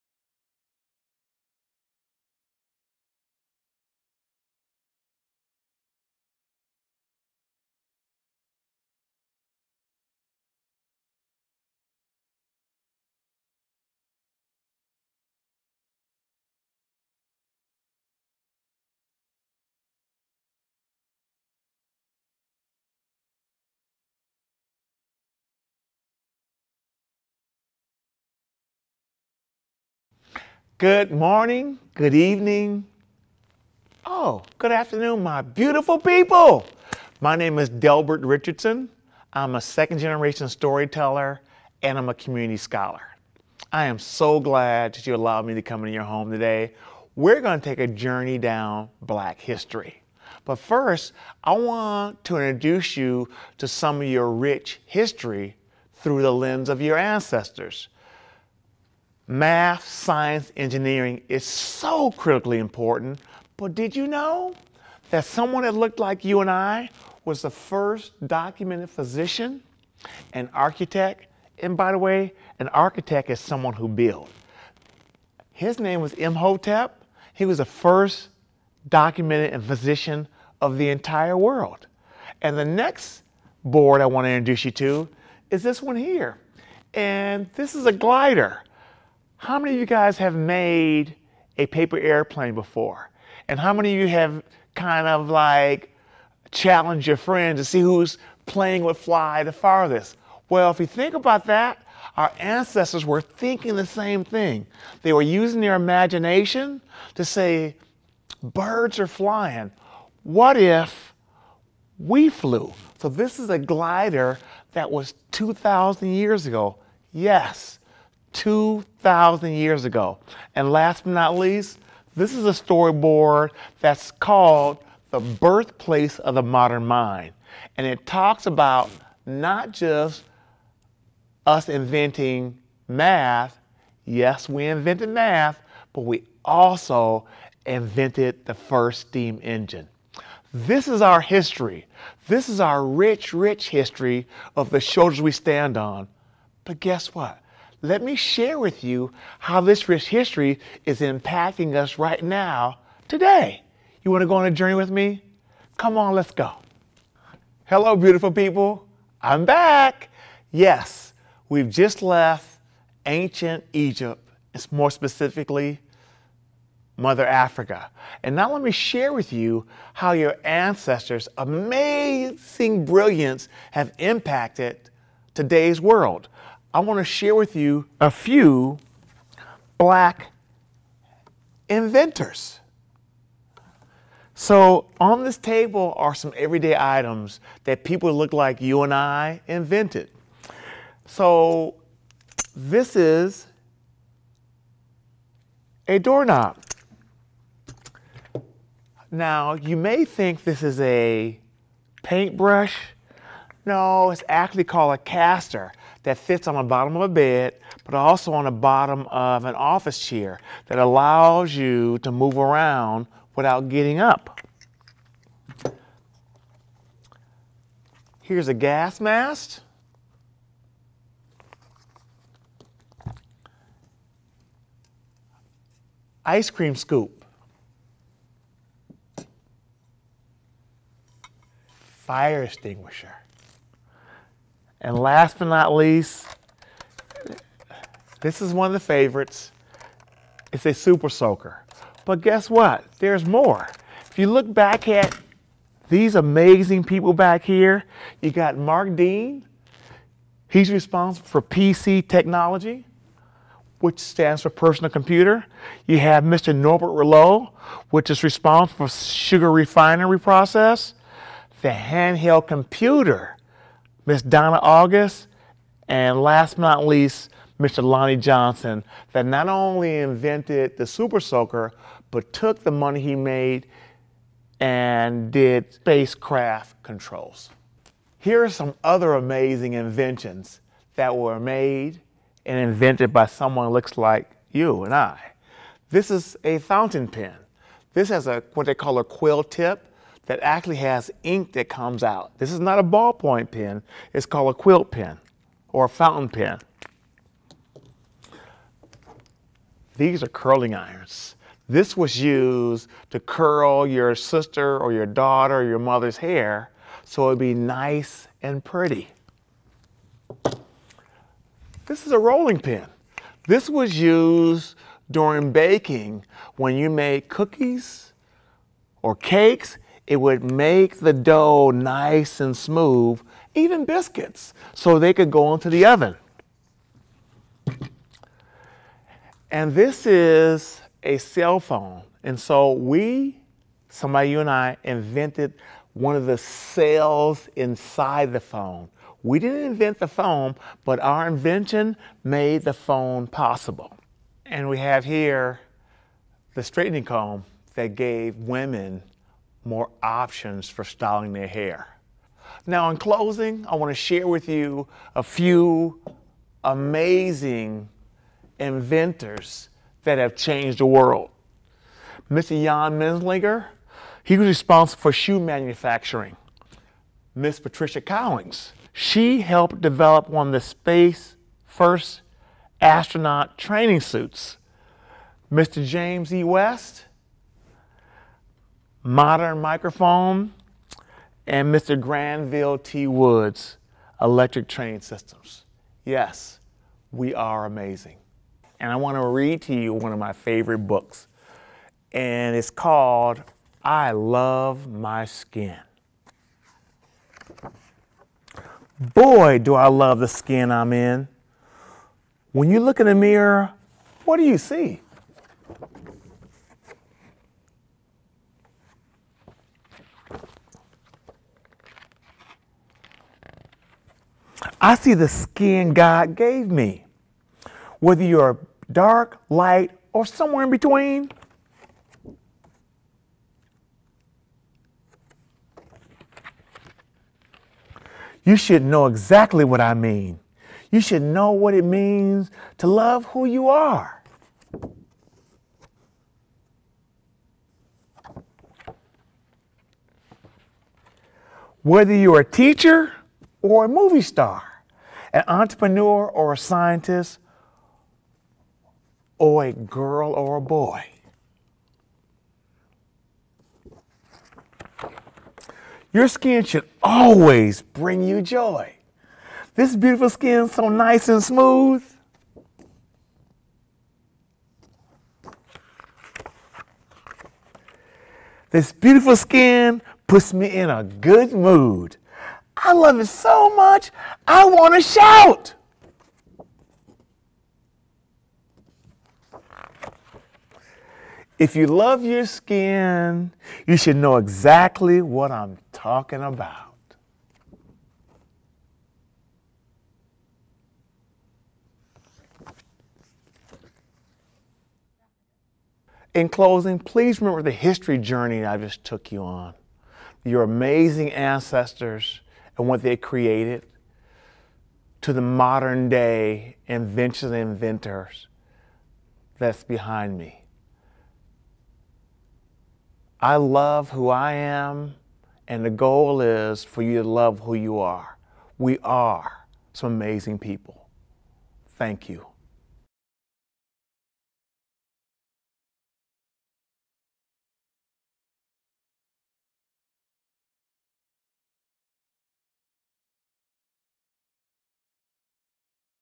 African American Museum - Delbert Richardson - Middle Elementary
This educational video focuses on the global histories and contributions of Africans from African, as well as Black/African Americans, specifically in the areas of S.T.E.A.M (Science, Technology, Engineering, Art, and Math) and Black inventors and inventions.